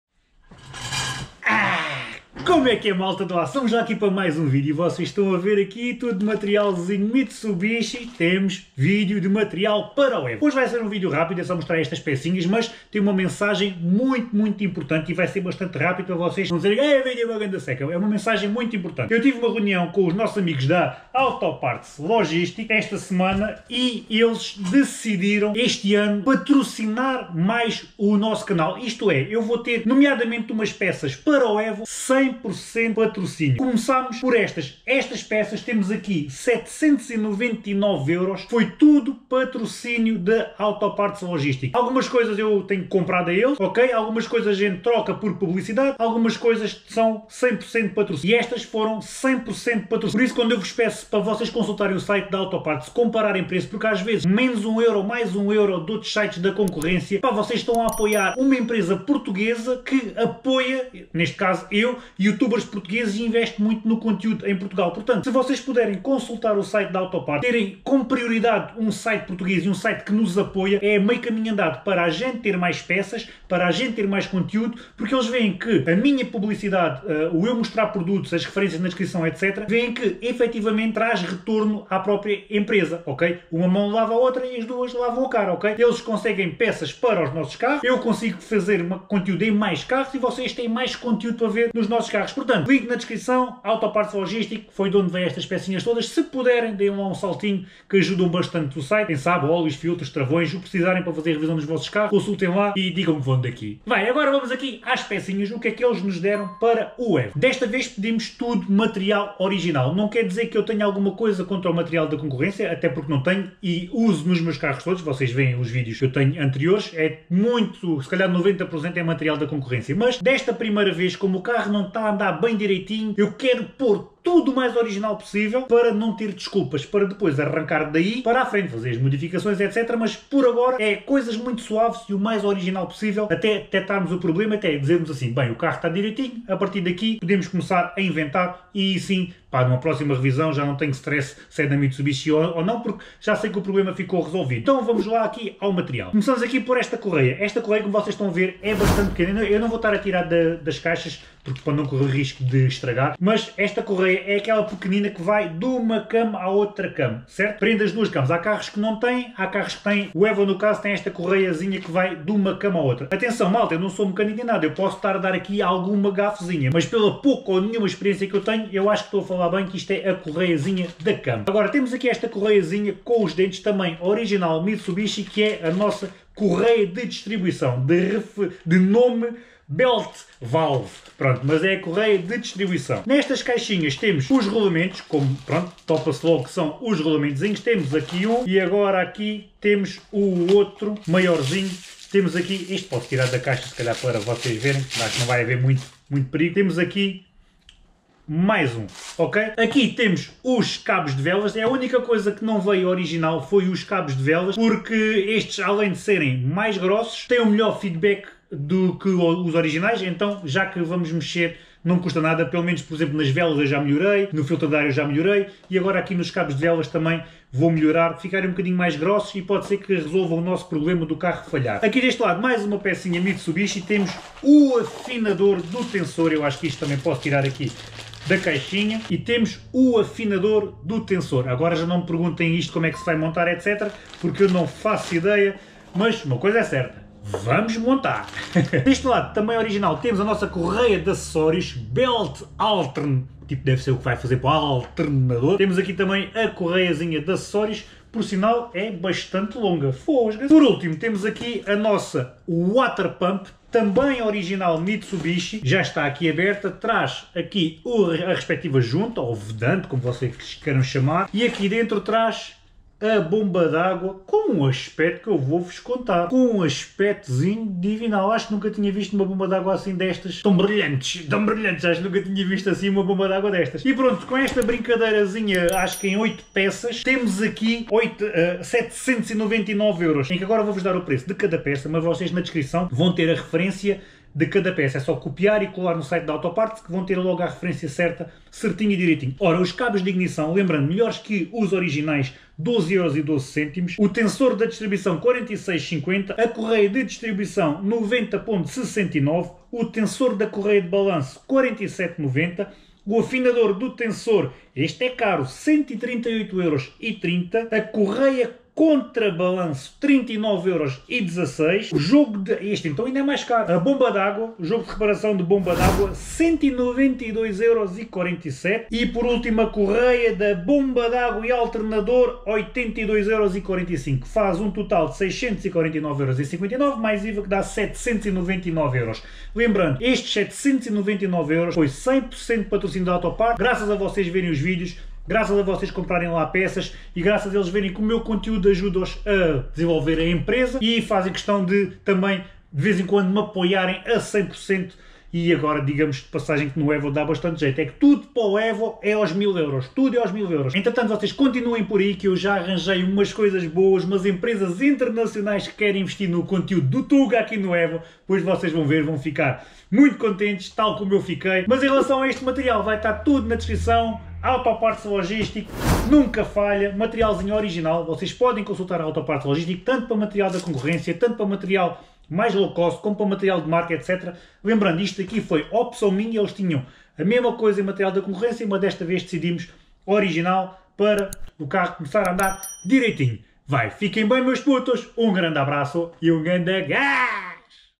Eles OFERECERAM-ME 799 euros em MATERIAL para o EVO 8
Material Original MITSUBISHI para comprar na AutoParts

Polie Correia Distribuição - REF - MD369999
Correia Distribuição - REF -  MD326059
Tensor correia contra balanço - REF -  MD352473
Afinador Tensor - REF -  MR984375
Correia Contra Balanço  - REF -  MR984778
Bomba Agua  - REF -  1300A065
Correia Acessorios  - REF -  MN158794
Cabos Velas - REF - IC-529